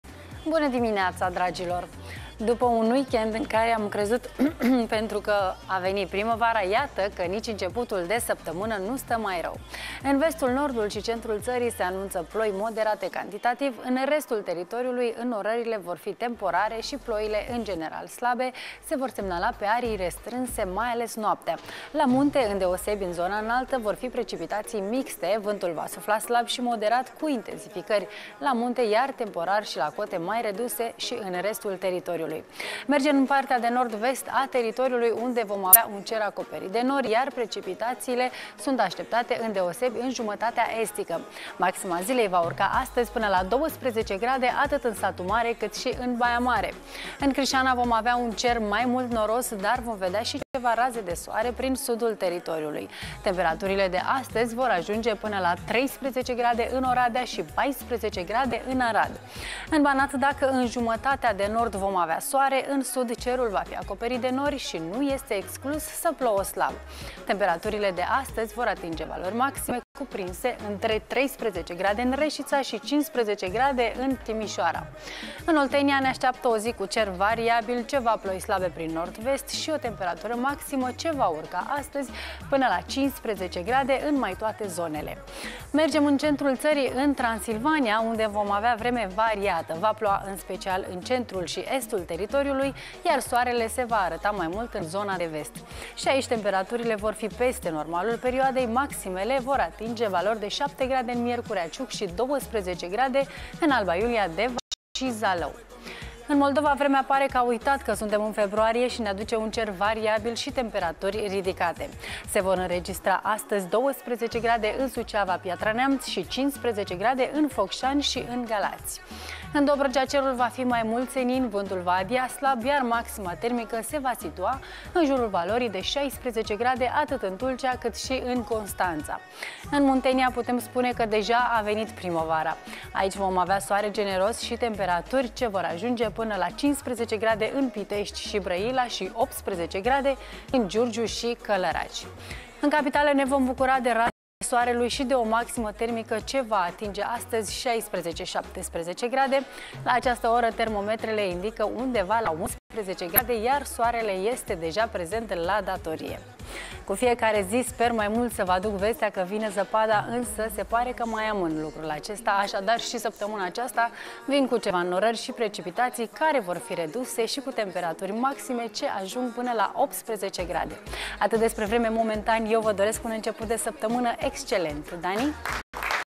Vremea se menţine neobişnuit de caldă, în continuare, în cea mai mare parte a ţării
După un weekend în care am crezut că a venit primăvara, iată că nici începutul de săptămână nu stă mai rău. În vestul, nordul şi centru ţării se anunţă ploi moderate cantitativ, iar în restul teritoriului înnorările vor fi temporare  cu ploi slabe.